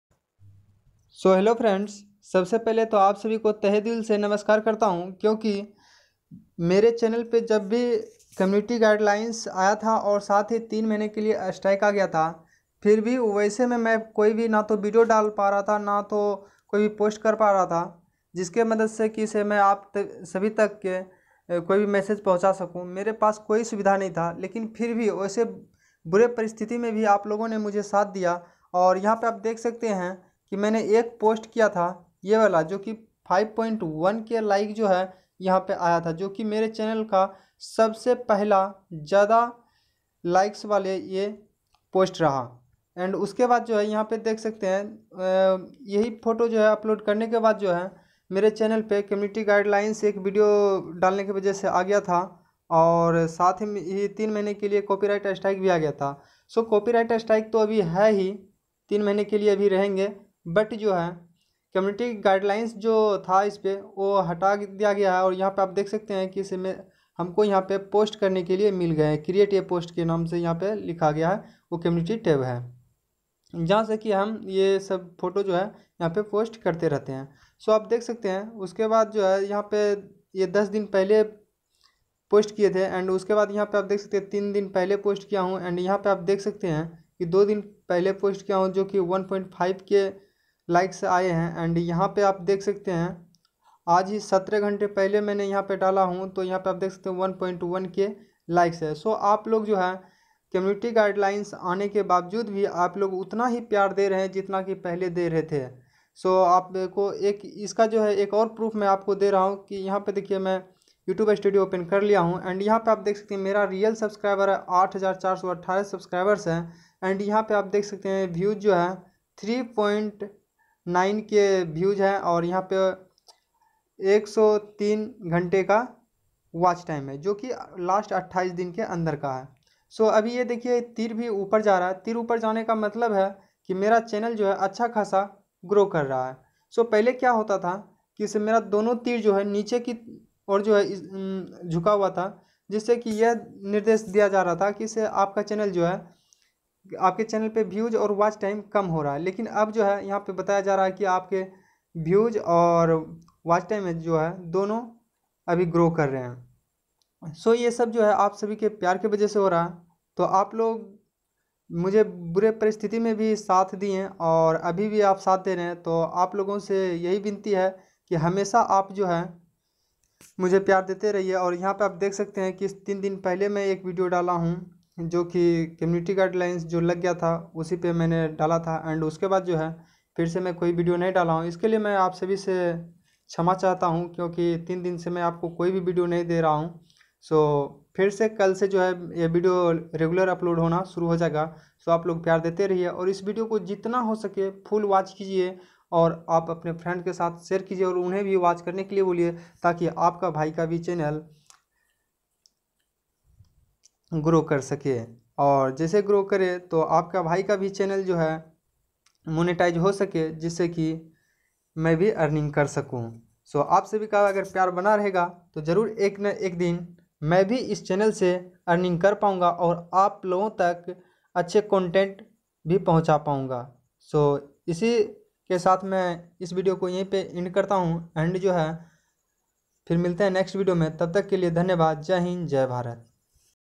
Community Guidelines Strike rule 2022 | Age resistance 18
Community Guidelines Strike rule 2022 | Age resistance 18 |  Video अपलोड नहीं हो रही है क्या करें |
hii friends aaj ki is video me hamne bataya hai ki maine video me kya galti kiya jo hame youtube copyright strike diya. so video pasand aaya ho to video ko like share and channel ko subscribe jarur kijiyega.

your questions
how to remove community guidelines strike 2022
community guidelines kaise hataye